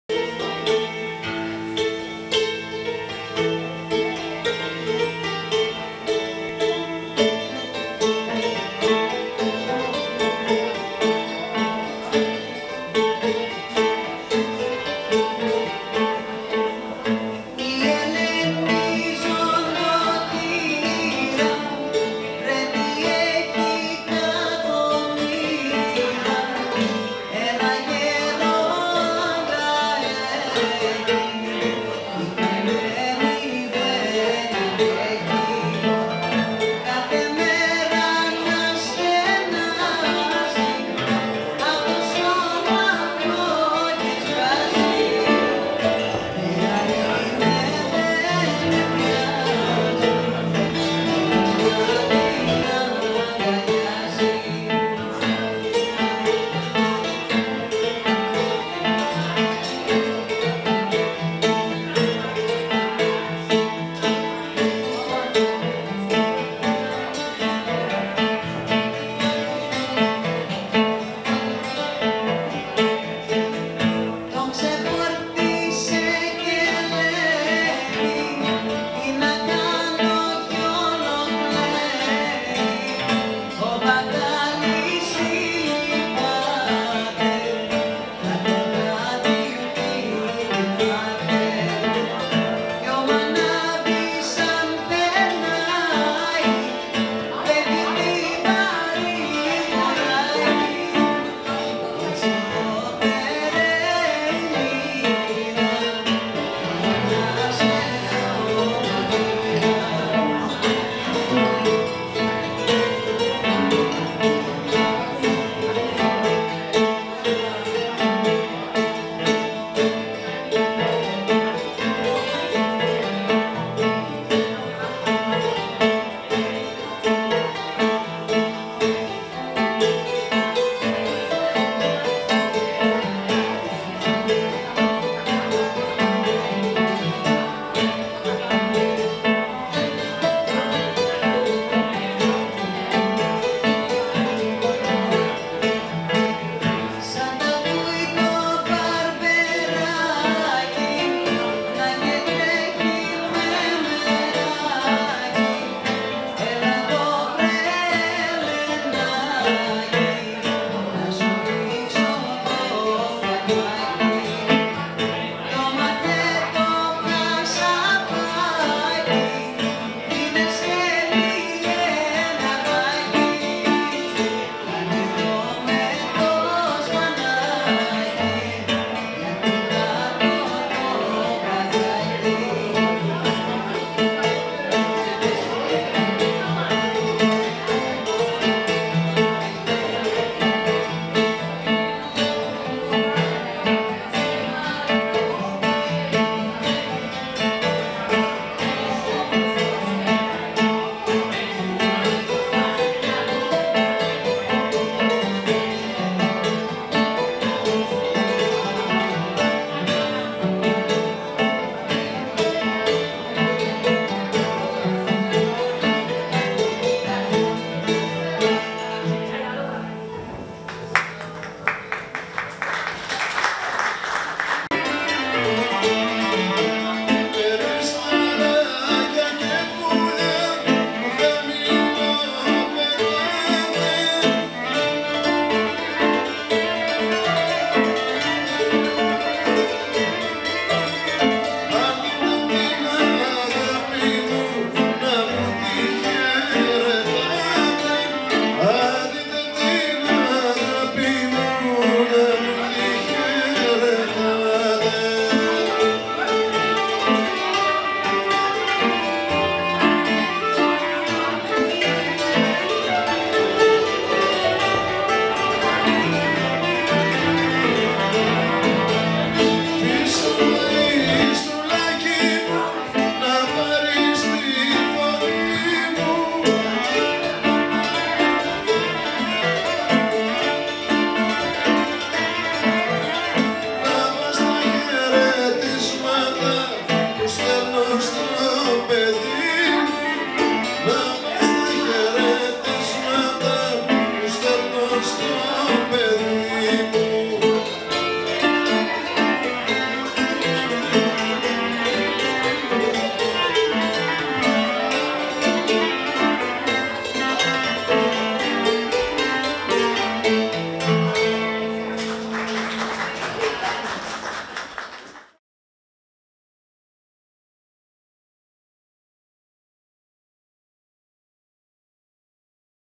MARIOS PAPADEAS...JIM KATSAROS VIDEO
LIVE AT THE " House Of Pan" Brighton Melbourne
16th of June 2018
vocals PASHALIA LATRAS